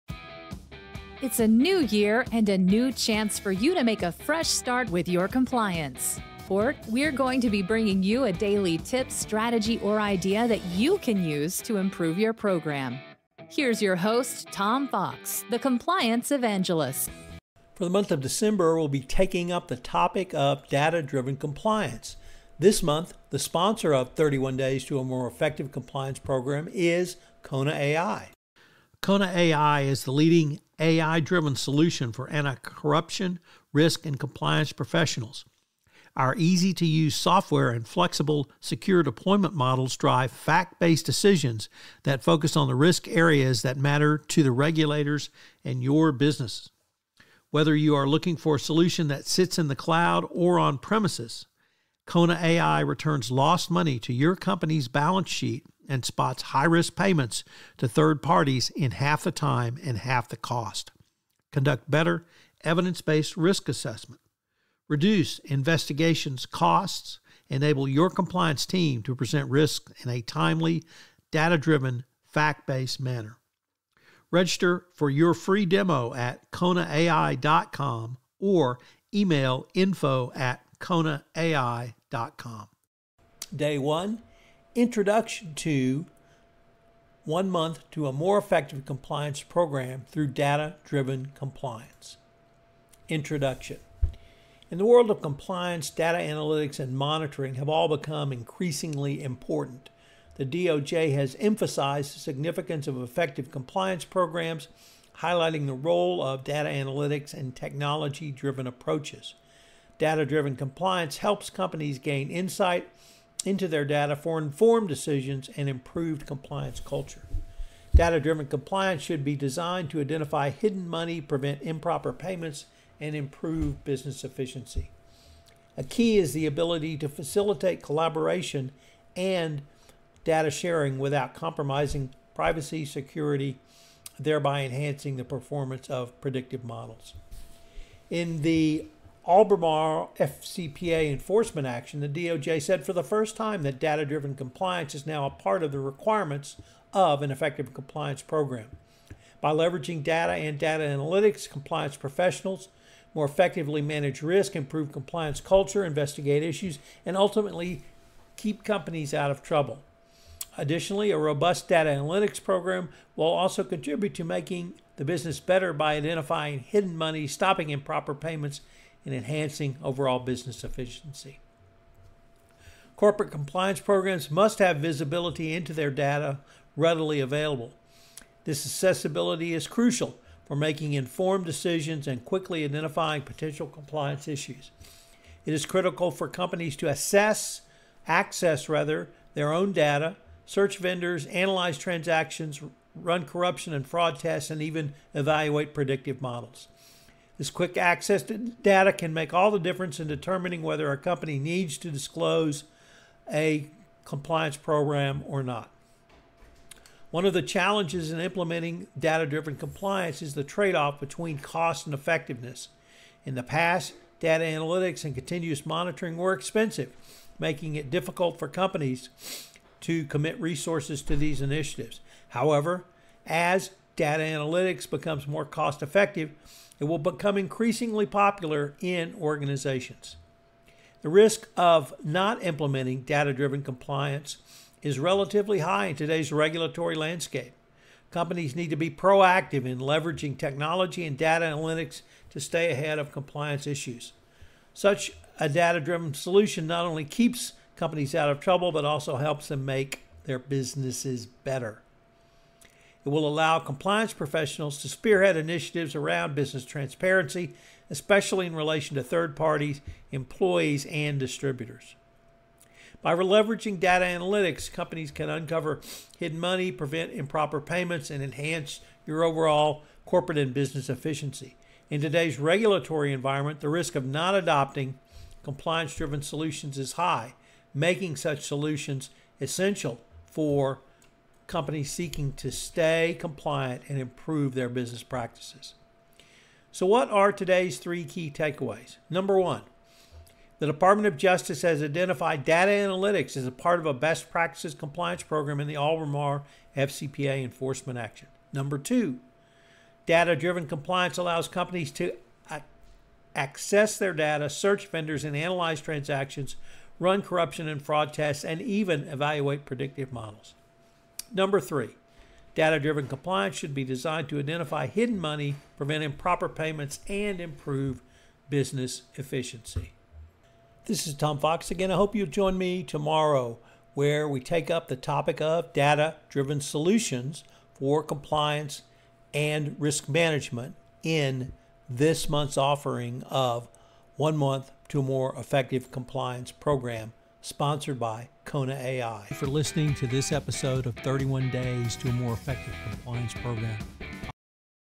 Driven Compliance: Day 1 – Introduction to Data – Driven Compliance
The Department of Justice has emphasized the importance of data analytics in compliance programs, aiming to identify hidden money, prevent improper payments, and improve business efficiency. This approach, facilitated by a robust data analytics platform, is crucial for compliance.

Three key takeaways:

1. The DOJ identified data analytics as a part of a best practices compliance program in the Albemarle FCPA enforcement action.

2. Data-driven compliance allows companies to access their data, search vendors, analyze transactions, run corruption and fraud tests, and even evaluate predictive models.

3. Data-driven compliance should be designed to identify hidden money, prevent improper payments, and improve business efficiency.